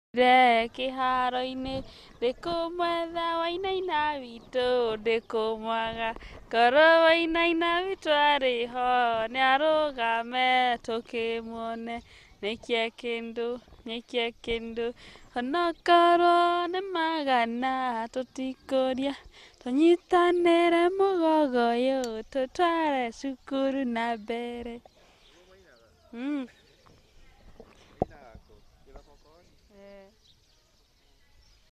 Jecintah Mutura
My Colleague Remembers a Harambee song during her time